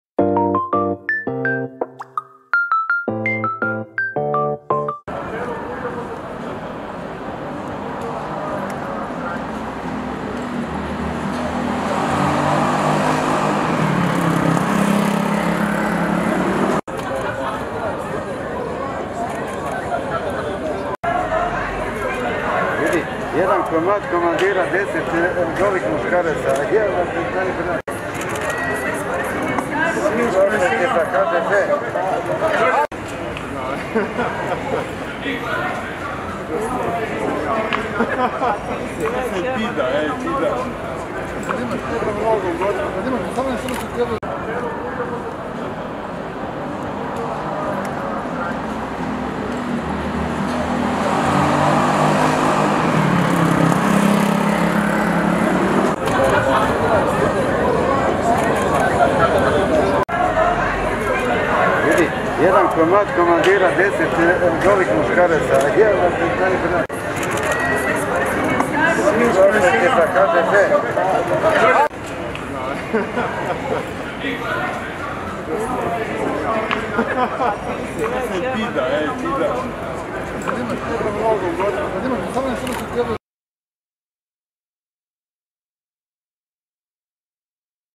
Hrvatski vaterpolisti u kupaćim gaćicama prošetali centrom Rijeke
Šetnja hrvatskih vaterpolskih reprezentativaca u kupaćim gaćicama i ogrtačima riječkim Korzom u sklopu snimanja promotivnog spota.